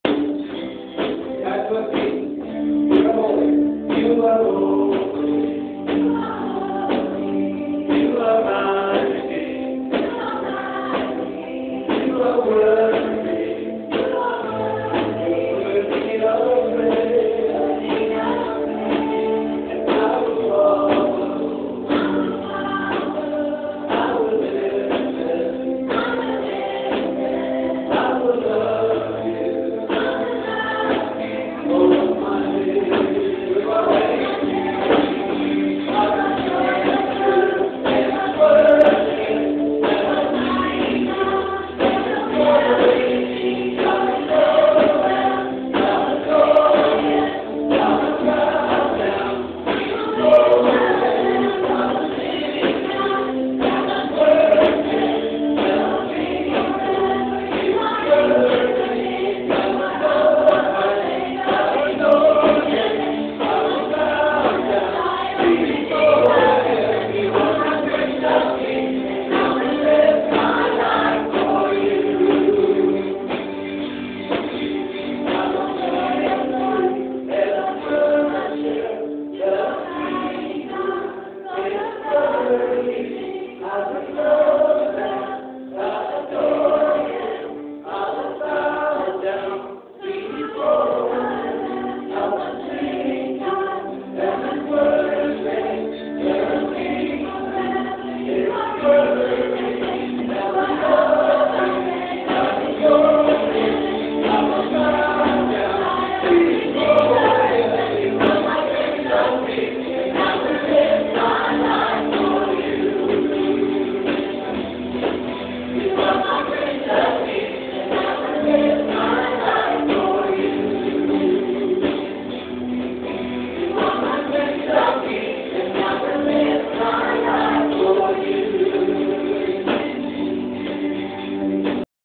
Sunday Morning Song
This is just one of my favorite Songs that they sing in our church on Sunday Morning.